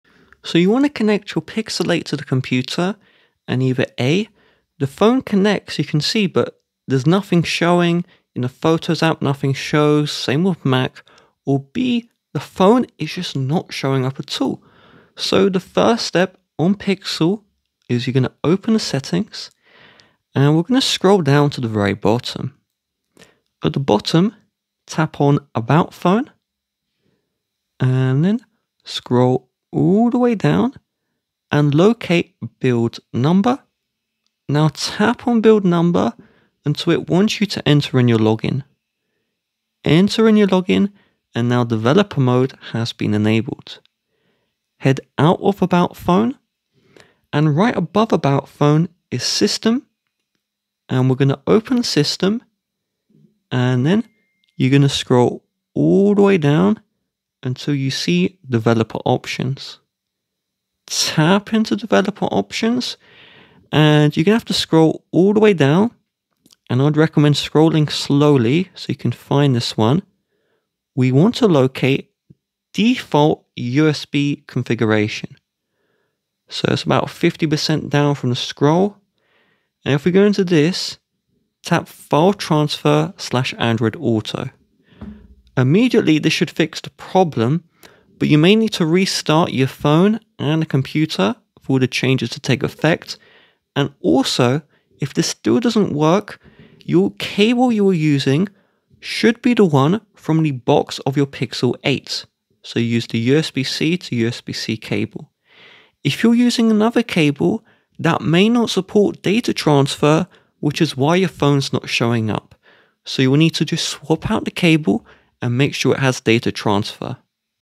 Fix Google Pixel 8 NOT Connecting To Computer
Welcome to our troubleshooting guide for fixing your Google Pixel 8 when it’s not connecting to your computer! In this video, we’ll walk you through effective solutions to resolve issues with USB connections and data transfer between your Pixel 8 and computer. Whether it’s a problem with drivers, settings, or cables, we’ll help you diagnose and fix the issue so you can seamlessly connect your Pixel 8 to your PC or Mac. Follow along to restore your connection and manage your data effortlessly!